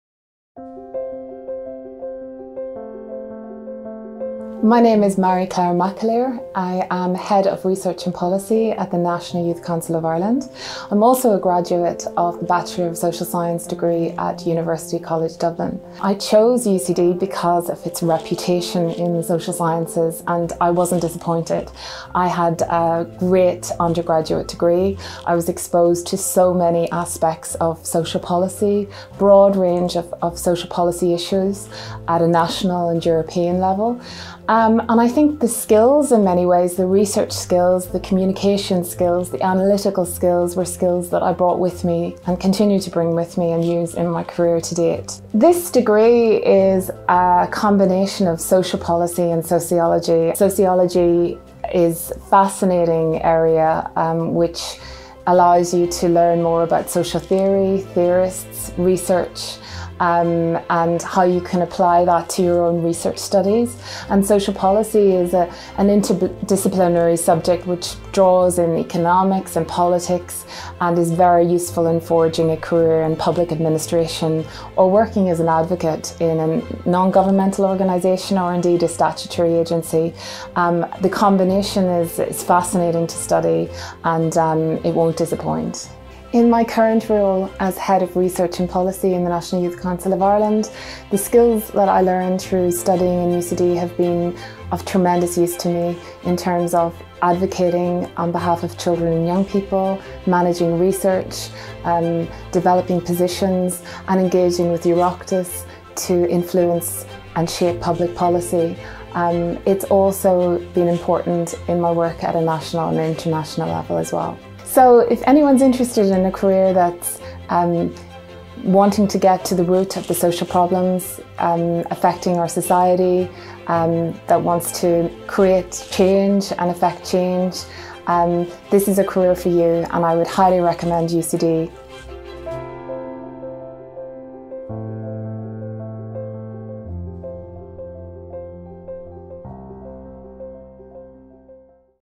Marie Claire McAleer - Social Policy & Sociology undergraduate degree
Hear from our Alumni about careers options available to Social Sciences graduates.